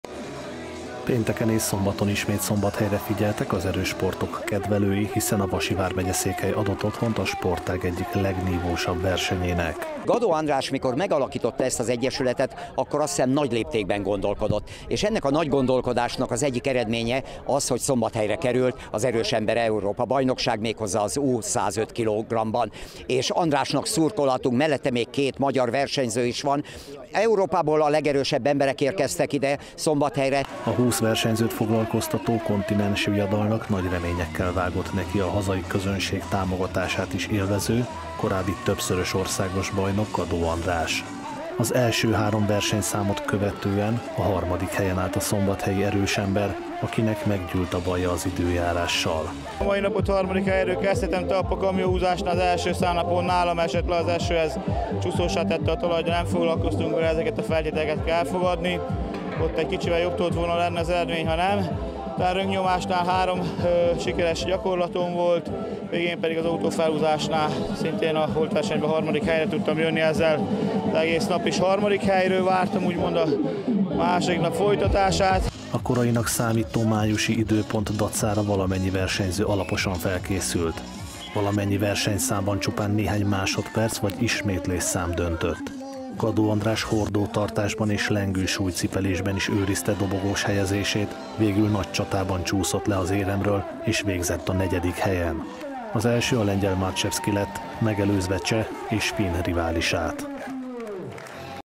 Európa legerősebb emberei látogattak Szombathelyre
Európa legerősebb emberei látogattak Szombathelyre, itt rendezték ugyanis az U105 kilogrammosok mezőny Európa-bajnokságát. A hazai színeket Gadó András képviselte, aki végig nagy csatában volt a bronzéremért, végül a negyedik helyet szerezte meg.